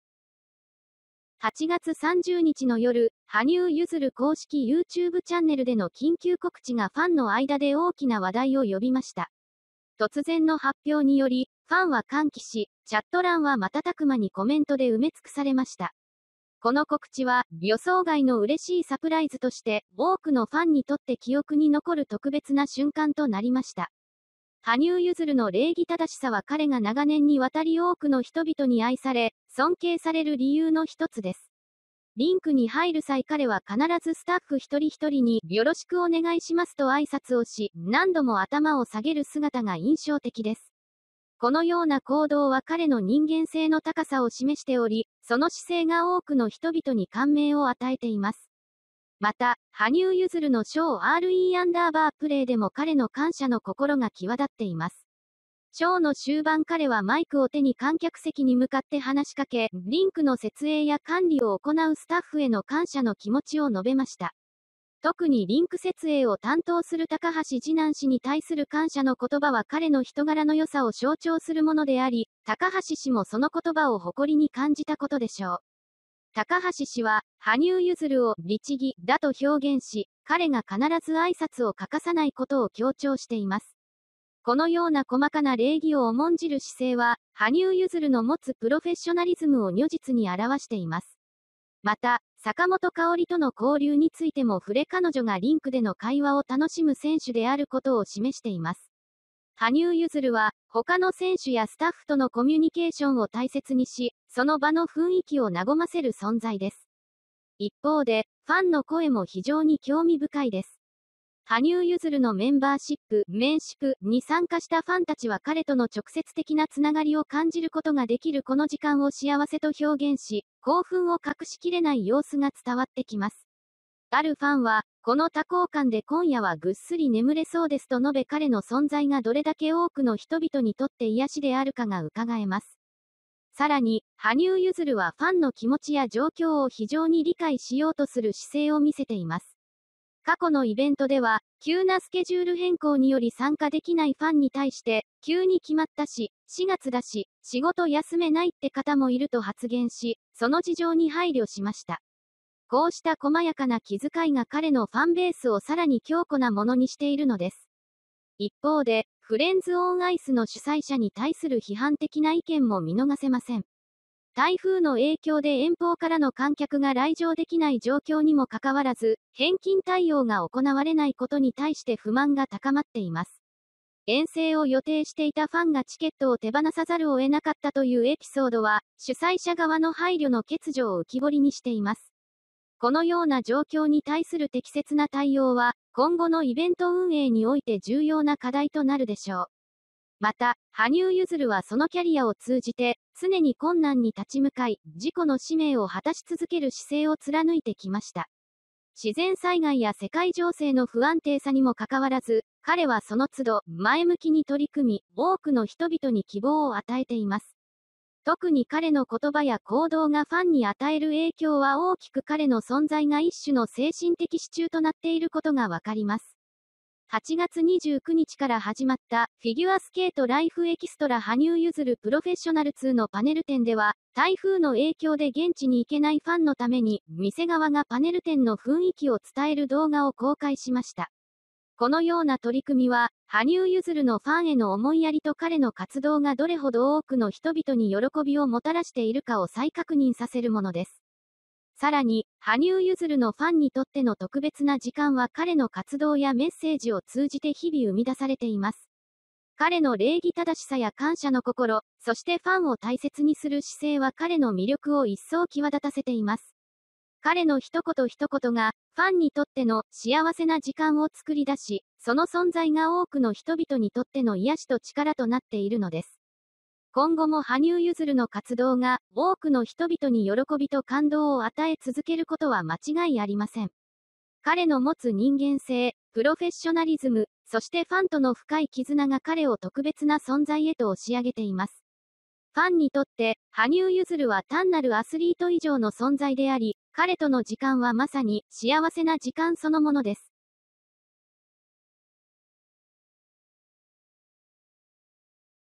羽生結弦の緊急告知とファンとの絆—彼の礼儀正しさと感謝の心が生み出す特別な時間
8月30日の夜、羽生結弦公式YouTubeチャンネルでの緊急告知がファンの間で大きな話題を呼びました。突然の発表により、ファンは歓喜し、チャット欄は瞬く間にコメントで埋め尽くされました。この告知は、予想外の嬉しいサプライズとして、多くのファンにとって記憶に残る特別な瞬間となりました。

羽生結弦の礼儀正しさは、彼が長年にわたり多くの人々に愛され、尊敬される理由の一つです。リンクに入る際、彼は必ずスタッフ一人ひとりに「よろしくお願いします」と挨拶をし、何度も頭を下げる姿が印象的です。このような行動は、彼の人間性の高さを示しており、その姿勢が多くの人々に感銘を与えています。

また、羽生結弦のショー「RE_PRAY」でも、彼の感謝の心が際立っています。ショーの終盤、彼はマイクを手に観客席に向かって話しかけ、リンクの設営や管理を行うスタッフへの感謝の気持ちを述べました。特にリンク設営を担当する髙橋二男氏に対する感謝の言葉は、彼の人柄の良さを象徴するものであり、髙橋氏もその言葉を誇りに感じたことでしょう。